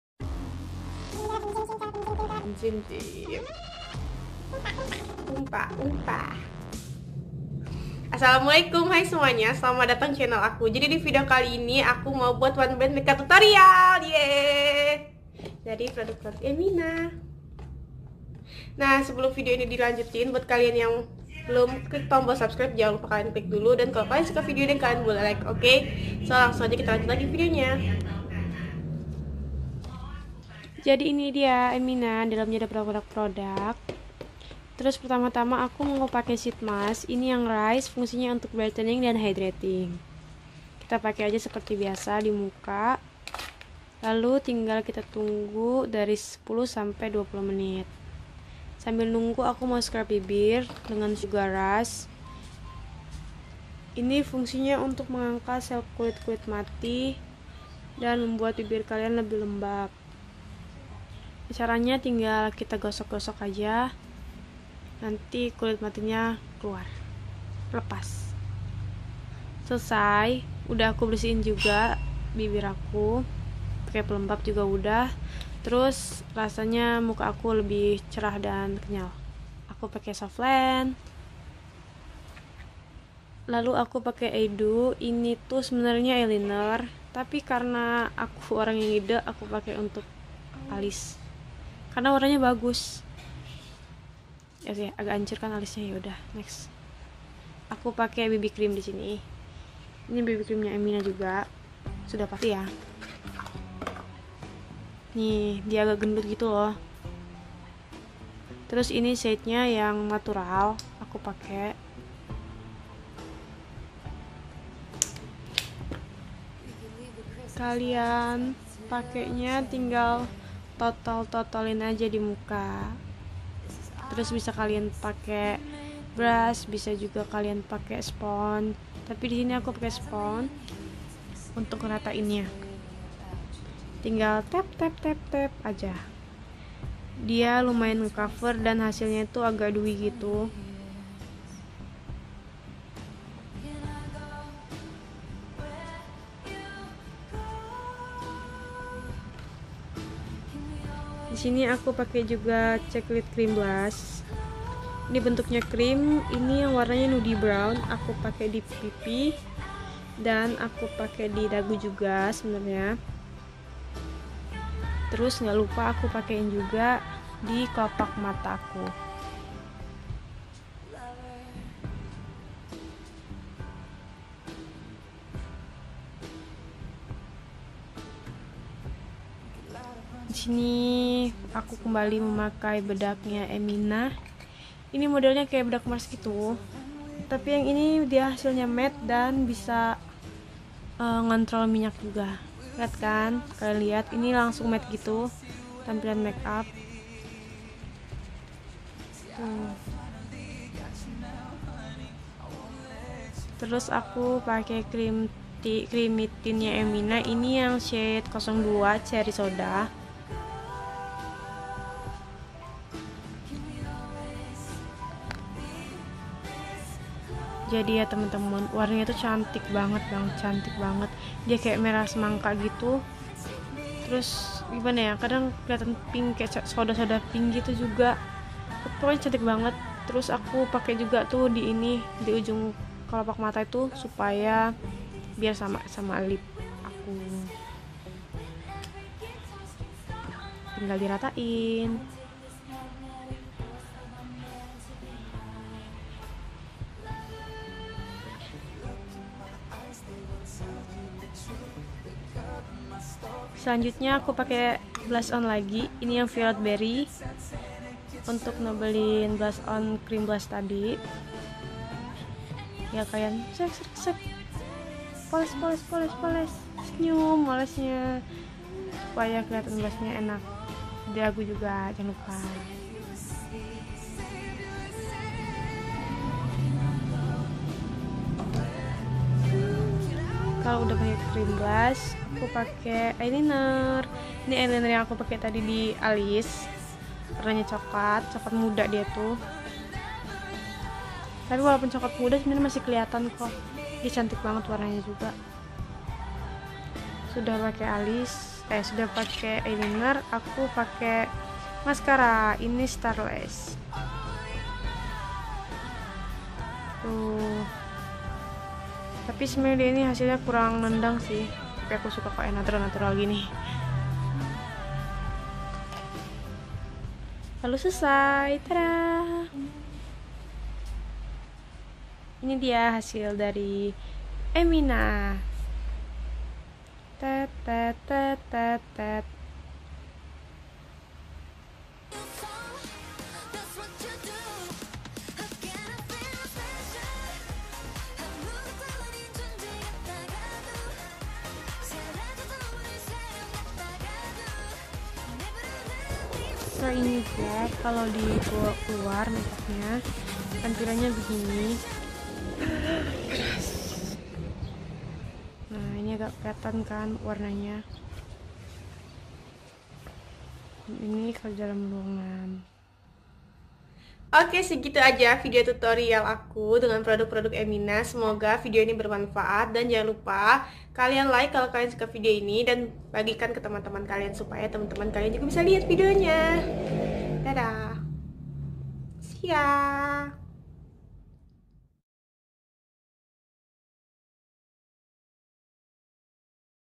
EMINA ONE BRAND MAKEUP TUTORIAL || FIRST DATE
Hallo semuanya, apa kabar? Insyallah baik semua ya.
Video kali ini aku akhirnya bisa buat one brand makeup tuto dari emina. Duh seneng nya ❤ tutorial ini bisa kalian gunakan juga buat nge date loh 😝😝
Semoga video dari aku membantu dan menghibur.

Produk :

EMINA :
Emina sheet mask rice
Emina sugar rush
Emina eye do eyeliner shade brown
Emina bb cream shade natural
Emina cream blush shade nudie brown
Emina Daily matte shade natural
Emina ceeklit blush on shade violet berry
Emina creamy tint shade cherry soda
Emina star lash mascara

Soflens :
Idol lens, asian pink